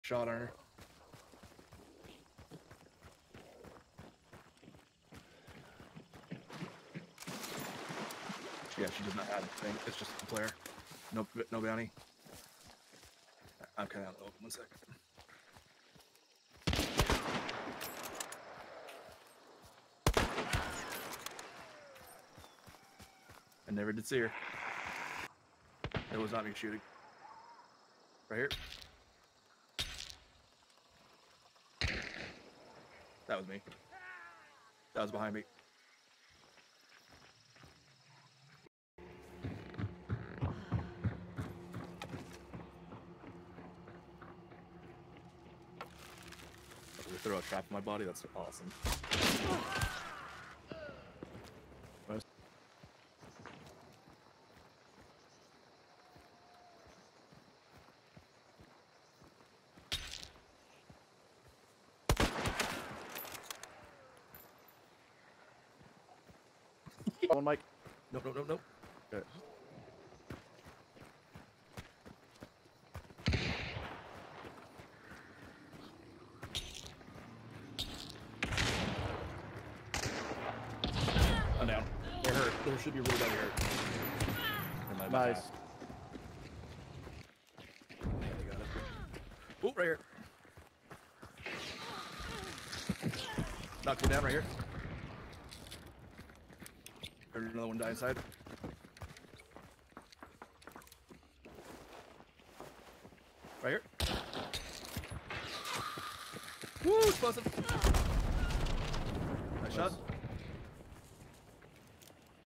Just a few kills with assorted weapons
My fave is probably the random death noise from the solo player I used a poison bolt on (blocking a successful self-res